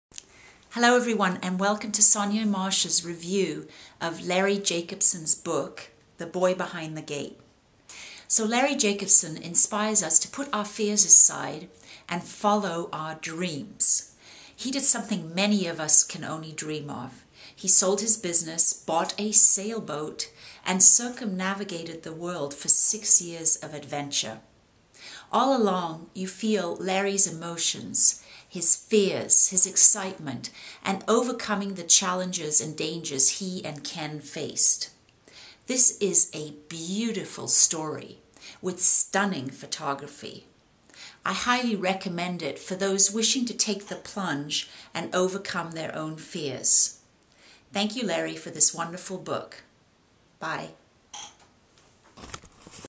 Review of Larry Jacobson's Book, The Boy Behind the Gate
A review of Larry Jacobson's book, "The Boy Behind the Gate."
A great book to inspire you to make your dreams happen.
Larry Jacobson inspires us to put our fears aside and follow our dreams. He did something many of us  can only dream of: he sold his business, bought a sailboat, and circumnavigated the world during his six year adventure. All along, you feel Larry's emotions: his fears, his excitement, and overcoming the challenges and dangers he and Ken faced. This is a beautiful story with stunning photography. I highly recommend it for those wishing to take the plunge and overcome their own fears.